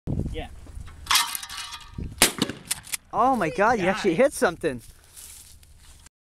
I got one
Me shooting clay pigeons with my brother and friend. And despite my buddy's tone I only missed a couple all afternoon.